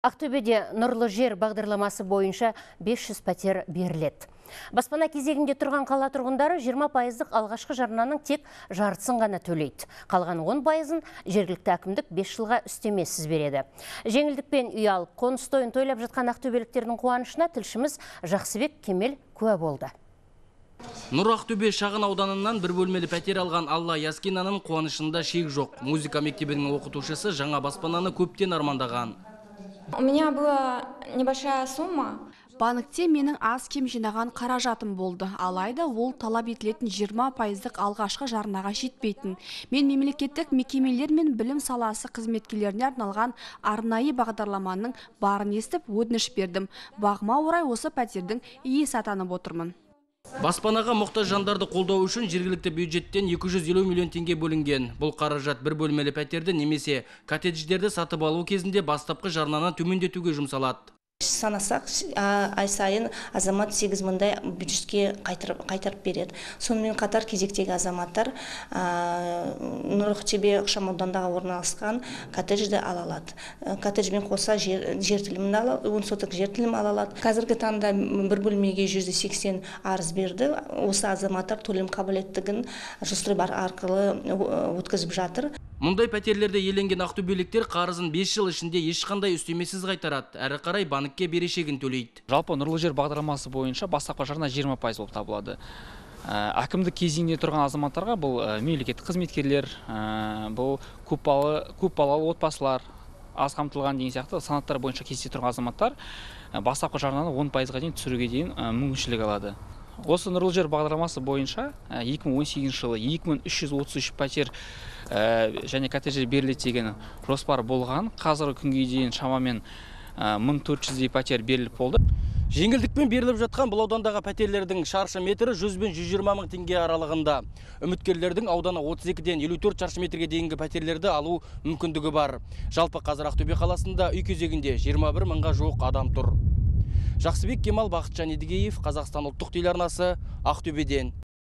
Ақтөбеде «Нұрлы жер» бағдарламасы бойынша 500 пәтер беріледі
Баспана кезегінде тұрған қала тұрғындары 20 пайыздық алғашқы жарнаның тек жартысын ғана төлейді. Қалған 10 пайызын жергілікті әкімдік 5 жылға үстемесіз береді.